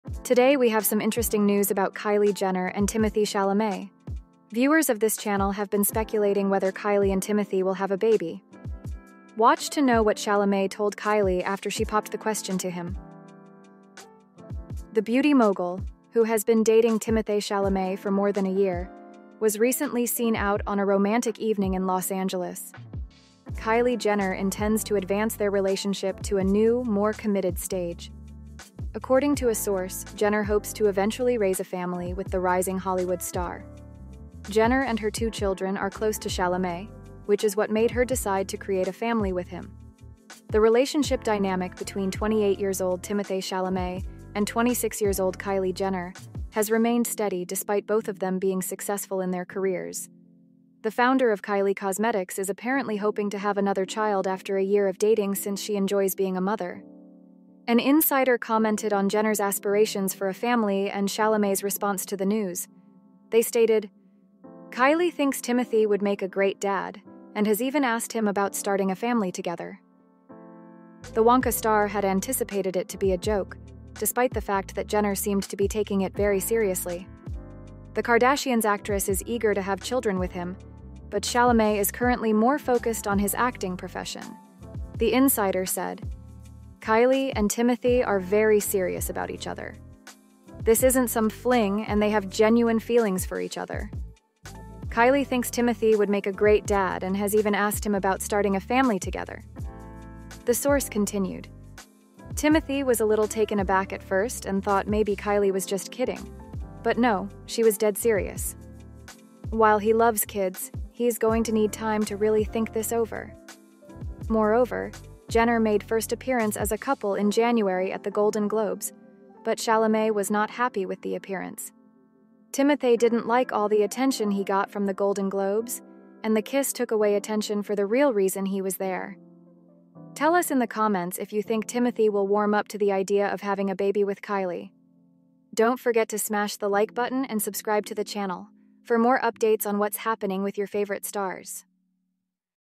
Kylie Jenner Committed To Having Kids With Timothee Chalamet
Kylie Jenner is said to be committed to having children with Timothee Chalamet in this celebrity gossip and entertainment news.
Kylie Jenner intends to advance their relationship with Timothee Chalamet to a new, more committed stage. The founder of Kylie Cosmetics is apparently hoping to have another child after a year of dating since she enjoys being a mother.